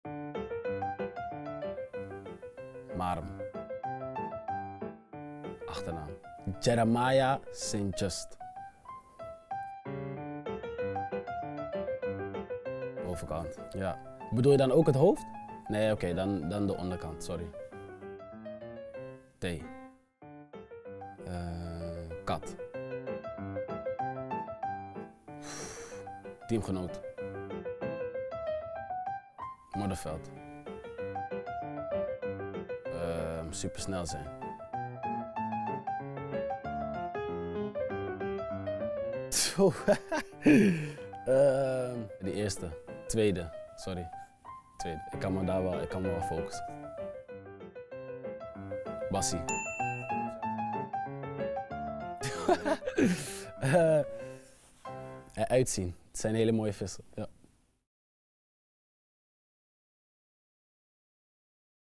Duivelsdilemma | Jeremiah St. Juste
In de rubriek Duivelsdilemma legt Feyenoord TV spelers pittige keuzes voor. Deze keer: Jeremiah St. Juste.